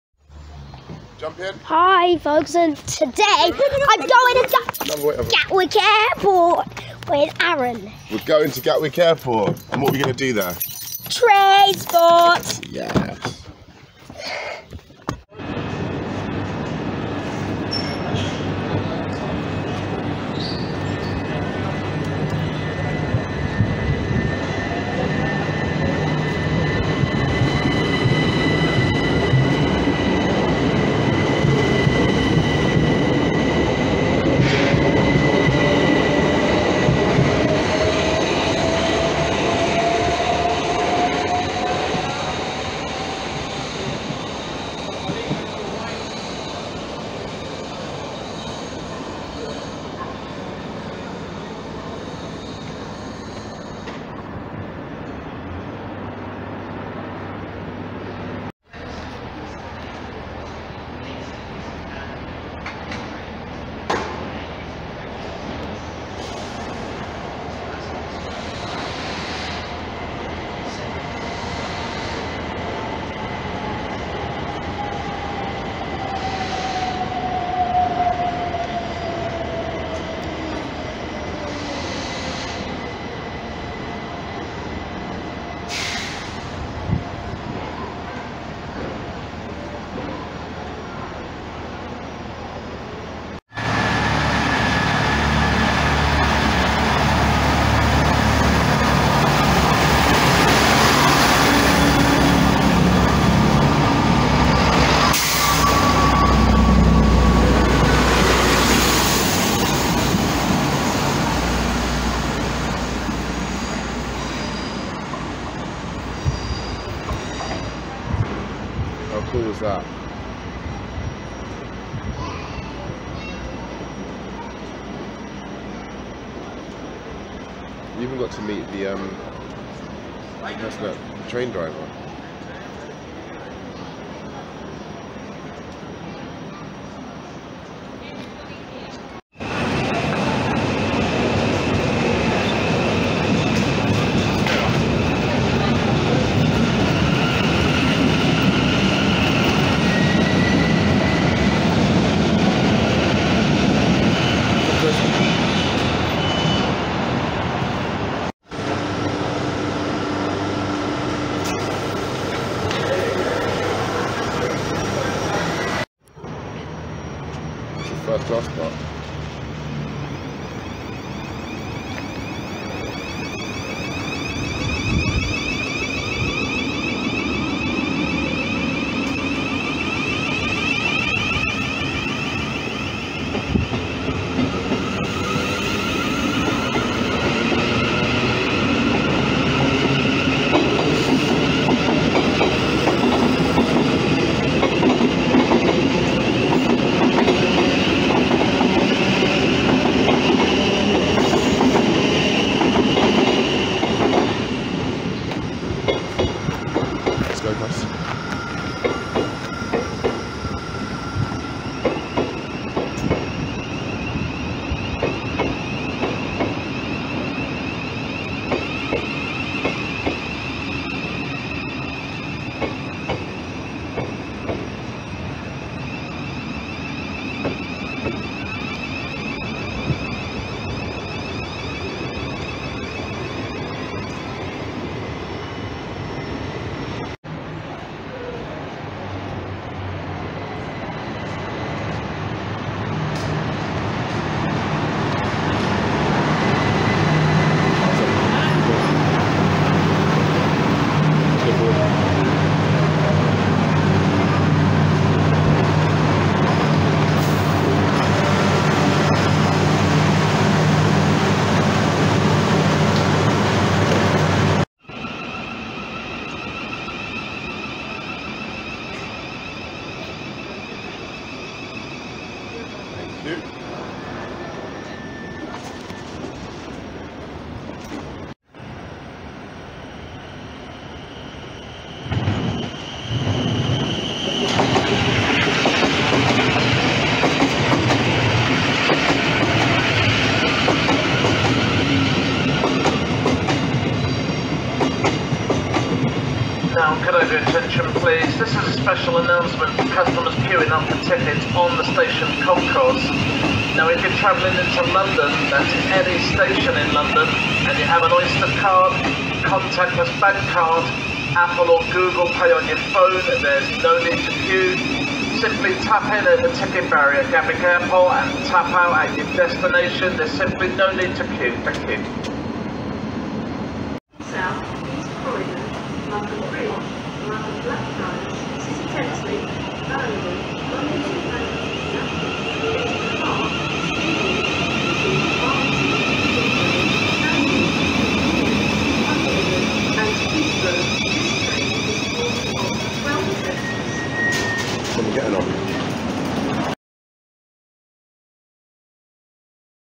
Trains at Gatwick Airport
Hello, I was so excited. I always wanted to go to Gatwick Airport, and I was lucky to have my big cousin take me this time.

I'm looking forward to my next adventure. Please continue having fun listening to and watching my videos until next time!